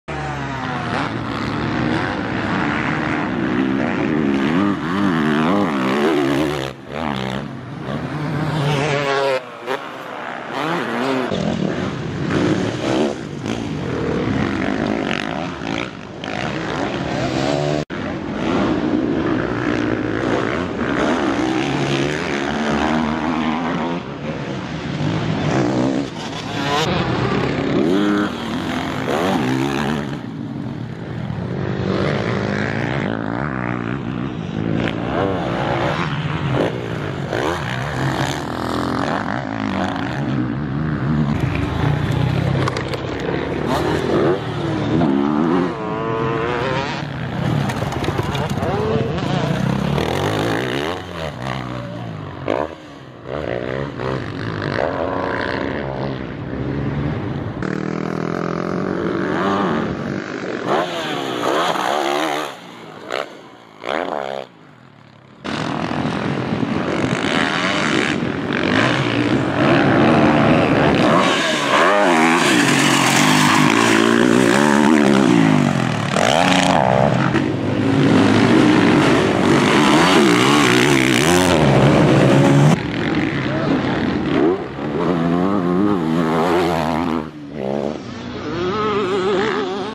Ian Oskar Katanec #928 | Training in Žiri
Ian Oskar Katanec #928 doing some hot laps in Športni park Jernejles.